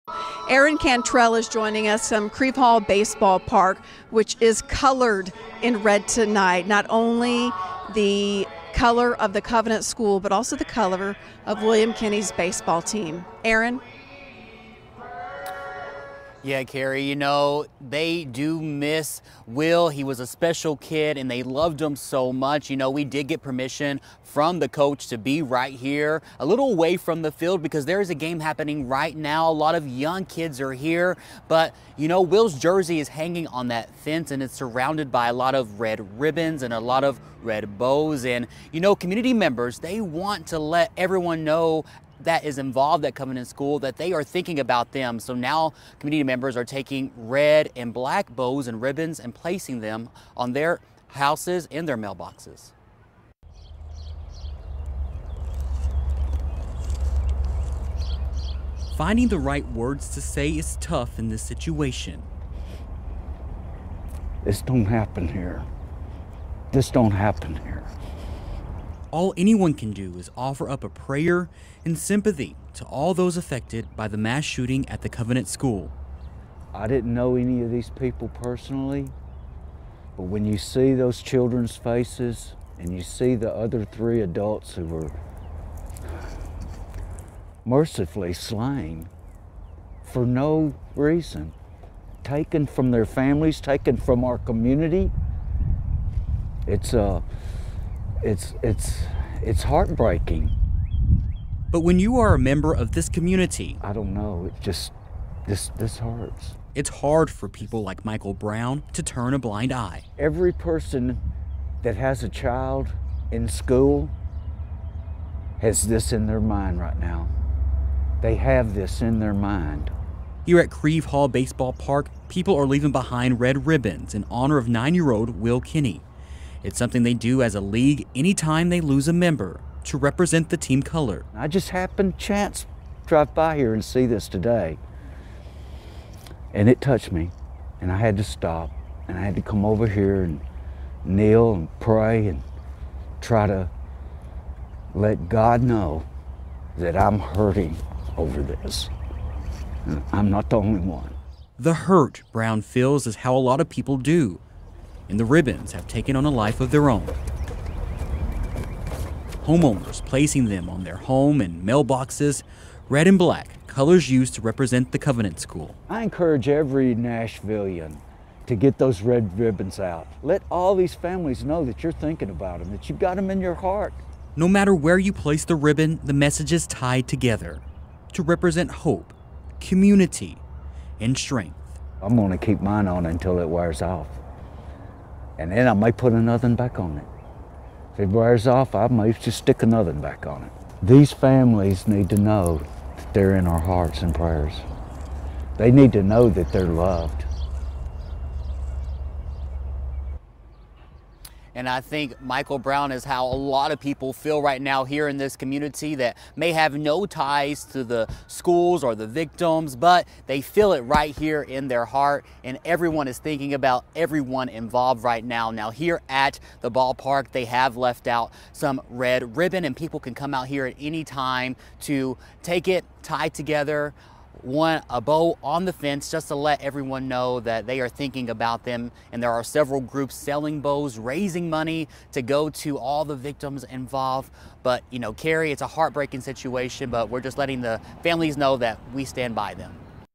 Tied together: Ribbons go up at the baseball field in honor of William Kinney
The red ribbons here at the Crieve Hall Baseball Park is a tradition the team does anytime they lose a player. They represent hope.